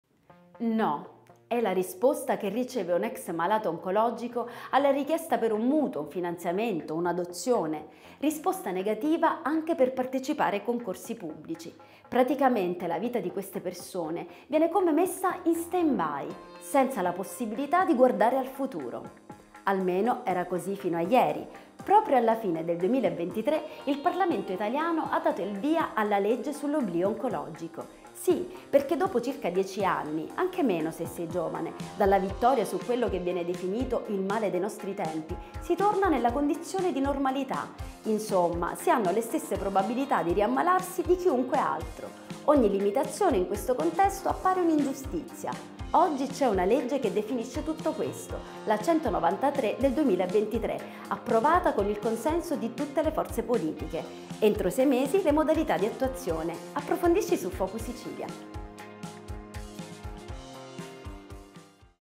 Oblio oncologico, la legge è in vigore “Effetti su prestiti, adozioni e concorsi”
"No"

❗È la risposta che riceve un ex malato oncologico alla richiesta per un mutuo, un finanziamento, una adozione. Risposta negativa anche per partecipare ai concorsi pubblici.

‼️Praticamente la vita di queste persone viene come messa in stand by senza la possibilità di guardare al futuro. Almeno era così fino a ieri. Proprio alla fine del 2023, il Parlamento italiano ha dato il via alla legge sull’oblio oncologico.

✔️Con la nuova norma dopo circa 10 anni (anche meno se sei giovane), dalla vittoria su quello che viene definito il male dei nostri tempi, si torna nella condizione di normalità. Insomma si hanno le stesse probabilità di ri ammalarsi di chiunque altro.

✔️Ogni limitazione in questo contesto appare una ingiustizia. Oggi c’è una legge che definisce tutto questo, la 193 del 2023, approvata con il consenso di tutte le forze politiche. Entro 6 mesi le modalità di attuazione

Cosa ne pensi?